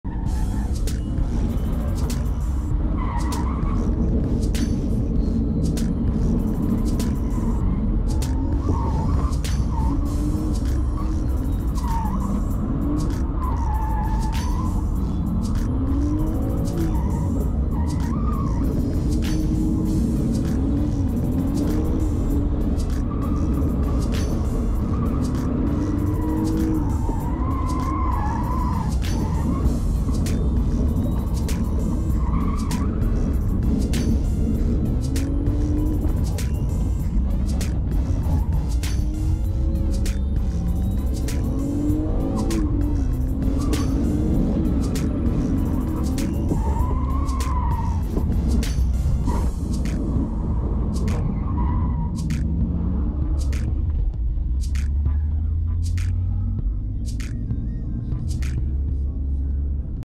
MissileMonday Drift.de with the New Car
2nd Lap in the new Car, new Suspension Geometry, new Angle Kit, new Fly Off Position, new everything...